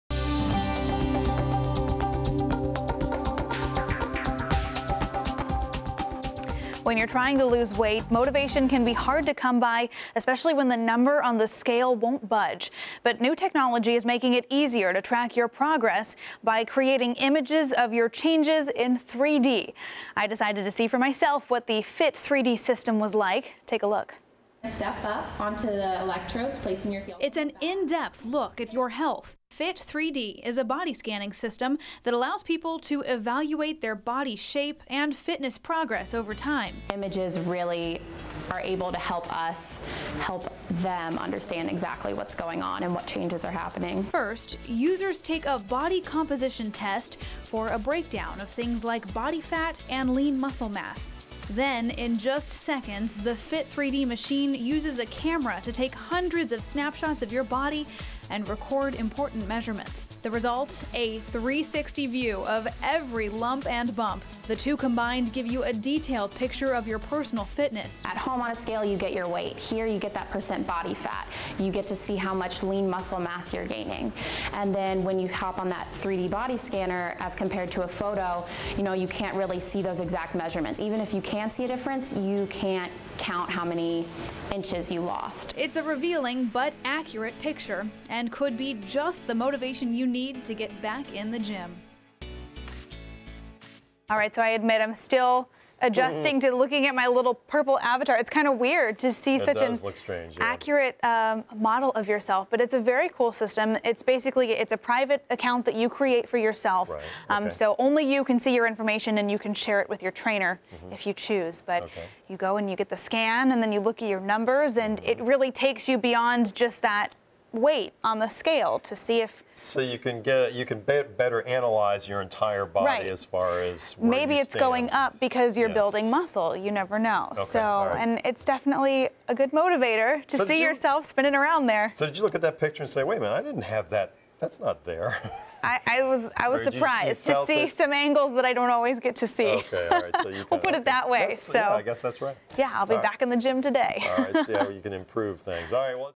Fit3D ProScanner Technology at GHF
The Fit3D ProScanner system, only available at Gainesville Health & Fitness, builds a personalized 3D model of your body and automatically captures the most important measurements in under a minute. Circumference, height, volume, and length measurements are captured and available on the secure Fit3D web platform, and can be shared with trainers, wellness coaches, and others.  A great way technology helps us get and stay motivated for the best results.